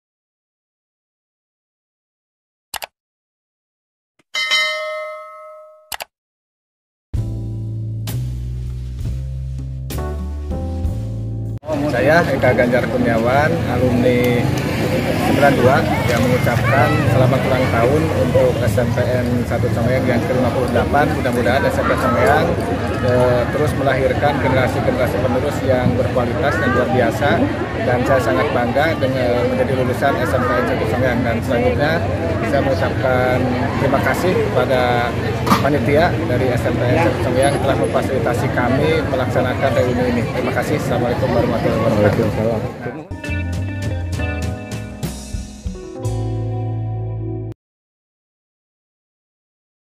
KANG EKA GANJAR : "SELAMAT ULANG TAHUN SMPN 1 CONGGEANG"
Bpk. Eka Ganjar Kurniawan, S.Sos.ME SekDis Pendidikan Kab. Sumedang yang juga Alumni 1992 SMPN 1 Conggeang , mengucapkan Selamat Ulang Tahun yang ke 58 SMPN 1 Conggeang

Be Smart Festival 2023
Milad Ke 58 & Reuni Akbar SMPN 1 Conggeang [NESCO]